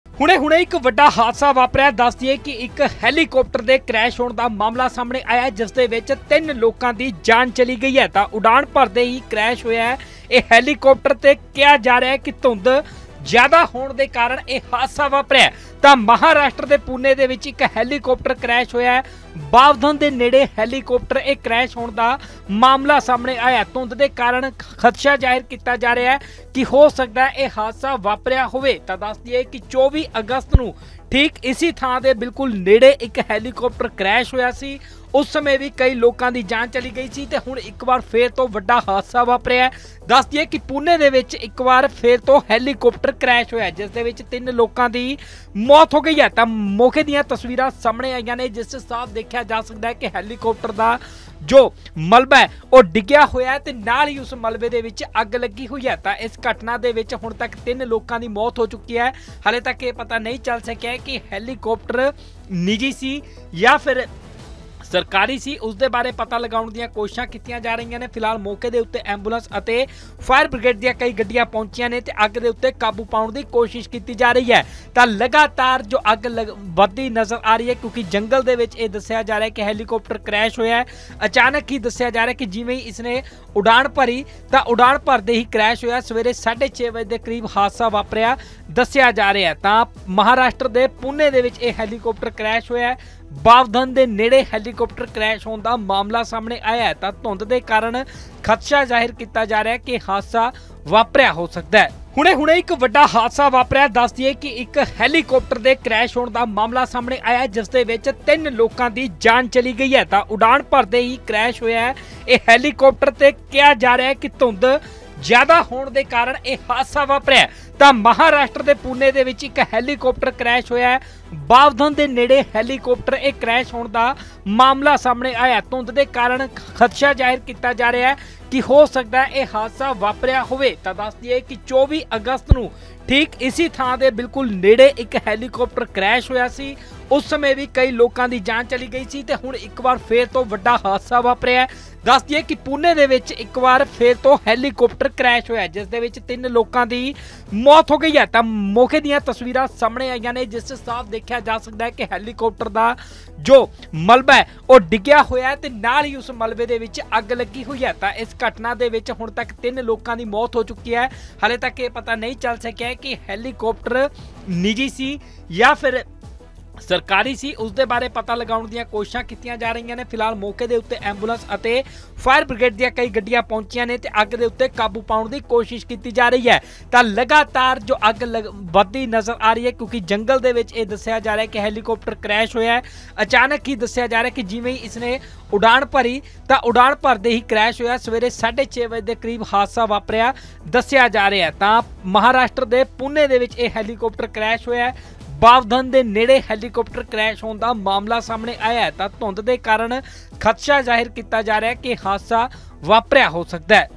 Helicopter crashes in Pune's Bavdhan | ਹੈਲੀਕਾਪਟਰ ਕ੍ਰੈਸ਼, ਹਾਦਸੇ 'ਚ 3 ਲੋਕਾਂ ਦੀ ਮੌਤ | Punjabi News|N18V
ਮਹਾਰਾਸ਼ਟਰ ਦੇ ਪੁਣੇ 'ਚ ਹੈਲੀਕਾਪਟਰ ਕ੍ਰੈਸ਼। ਬਾਵਧਨ ਦੇ ਨੇੜੇ ਹੈਲੀਕਾਪਟਰ ਹੋਇਆ ਕ੍ਰੈਸ਼। ਹਾਦਸੇ 'ਚ 3 ਲੋਕਾਂ ਦੀ ਮੌਤ। ਉਡਾਣ ਭਰਦੇ ਹੀ ਕ੍ਰੈਸ਼ ਹੋਇਆ ਹੈਲੀਕਾਪਟਰ। ਧੁੰਦ ਕਾਰਨ ਹਾਦਸੇ ਦਾ ਖਦਸ਼ਾ। ਮੌਕੇ 'ਤੇ ਦੋ ਐਂਬੂਲੈਂਸ ਅਤੇ ਚਾਰ ਫਾਇਰ ਟੈਂਡਰ ਮੌਜੂਦ।